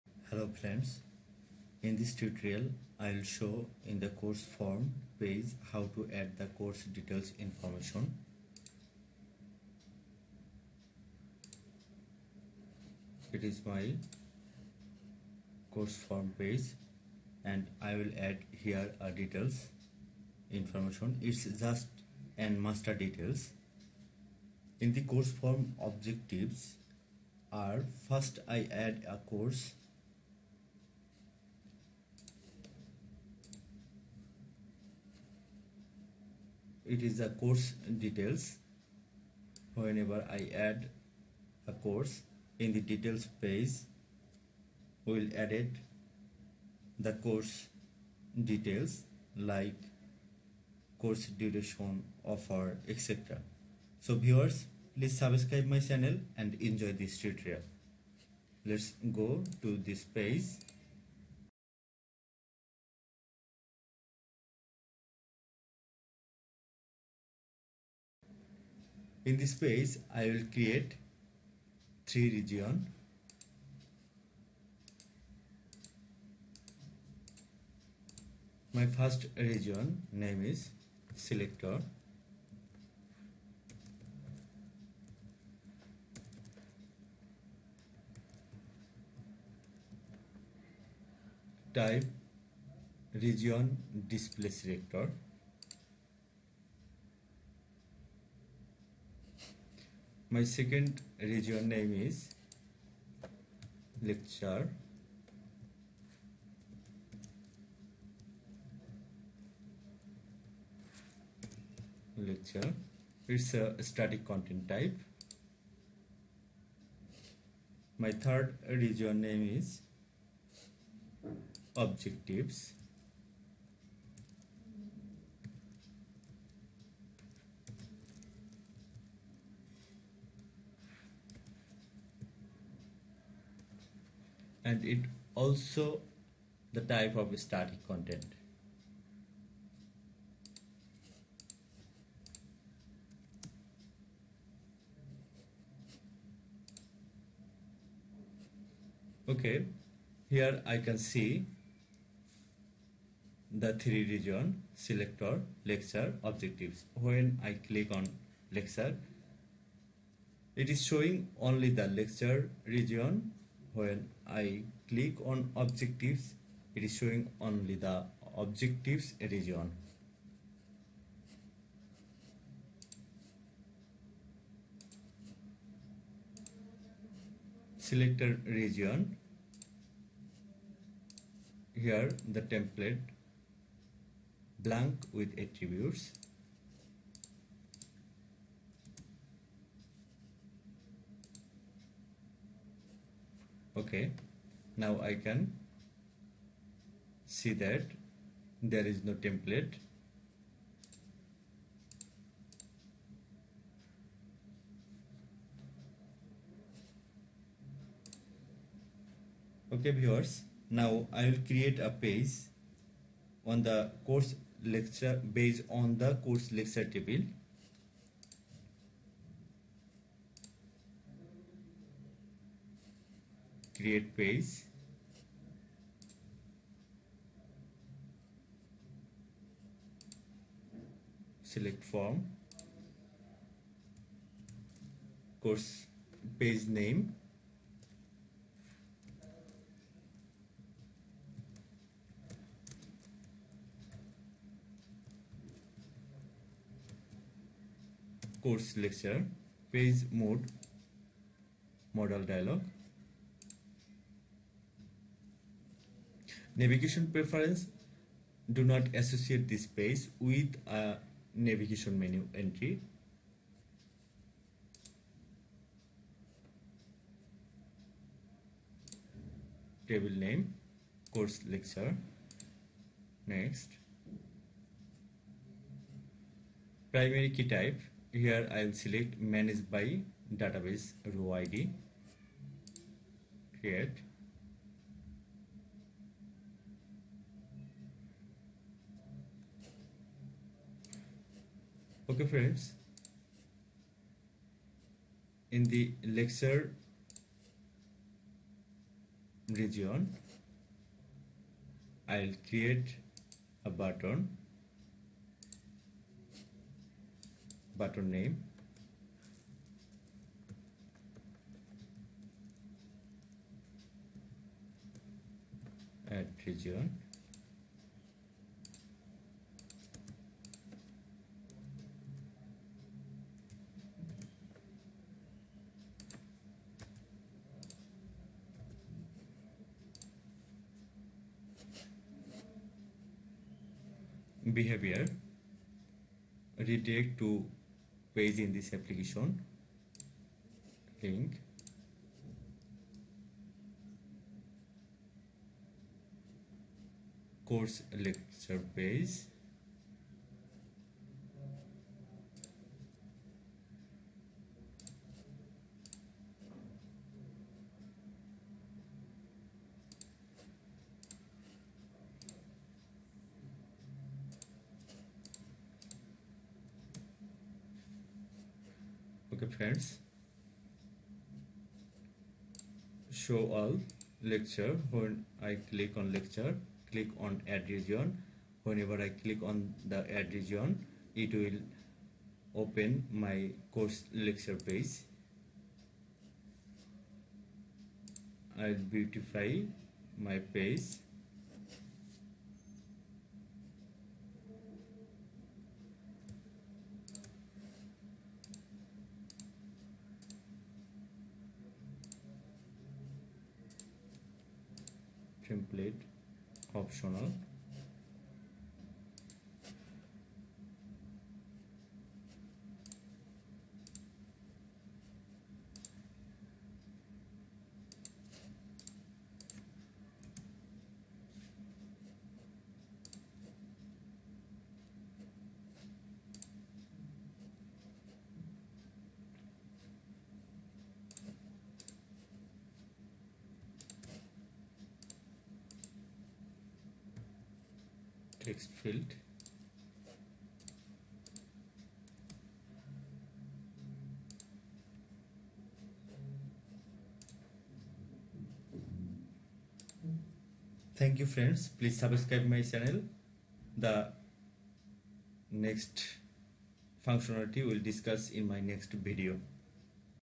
How to create Region Display Selector
This Video is The Series Video's 19th Video for developing a website With Oracle Apex By Baizeed Rony.